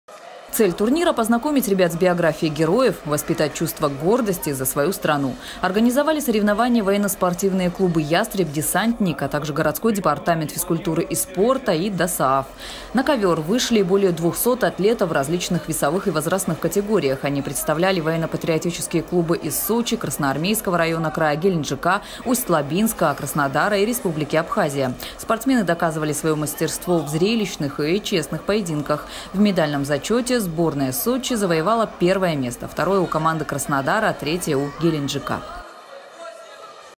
В Сочи турнир по рукопашному бою посвятили погибшему бойцу спецназа. Новости Эфкате
В Сочи почтили память Героя России Евгения Эпова. Молодой боец спецназа погиб в ходе спецоперации, но спас жизни своих товарищей ценой собственной. Памяти Эпова посвятили традиционный турнир по армейскому рукопашному бою.

На ковер вышли более 200 атлетов в различных весовых и возрастных категориях. Они представляли военно-патриотические клубы из Сочи, Красноармейского района края, Геленджика, Усть-Лабинска, Краснодара и Республики Абхазия. Спортсмены доказывали своё мастерство в зрелищных и честных поединках. В медальном зачете сборная Сочи завоевала первое место, второе - у команды Краснодара, третье - у Геленджика.

Цель турнира - познакомить ребят с биографией героев, воспитать чувство гордости за свою страну. Организовали соревнования военно-спортивные клубы "Ястреб, "Десантник", а также городской Департамент физкультуры и спорта и ДОСААФ.